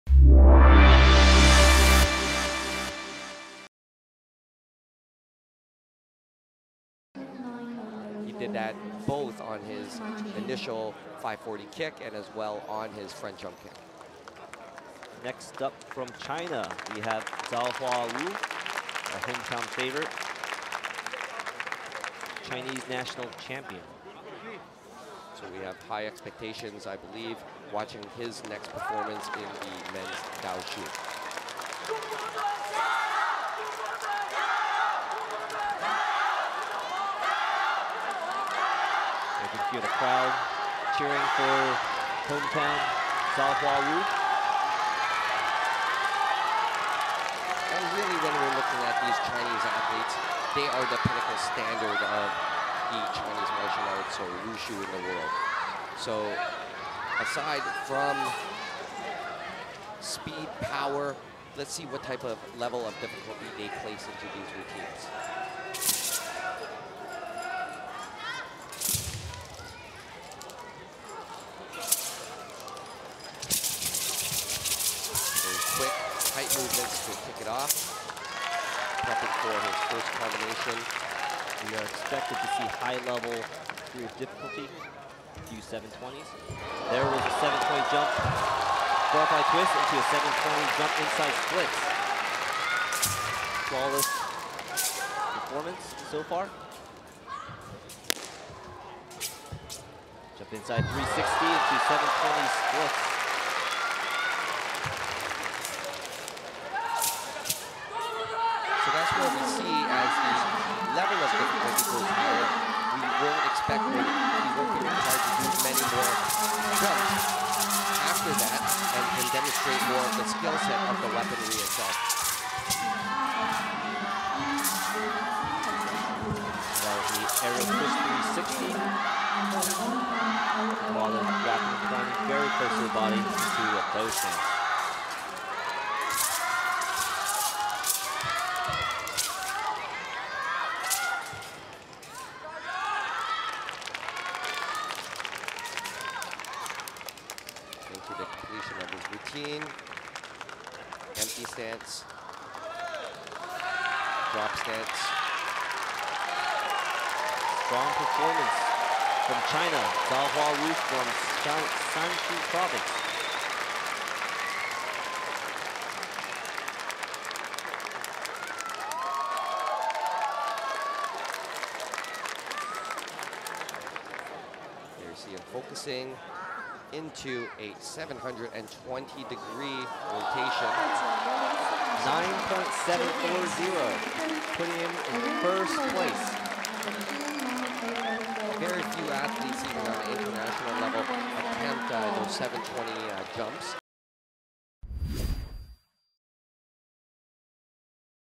15th WWC Men's Daoshu--1st Place (Gold)-Zhao Hua WU（CHN）
15th WWC Men's Daoshu--1st Place (Gold)-Zhao Hua WU（CHN）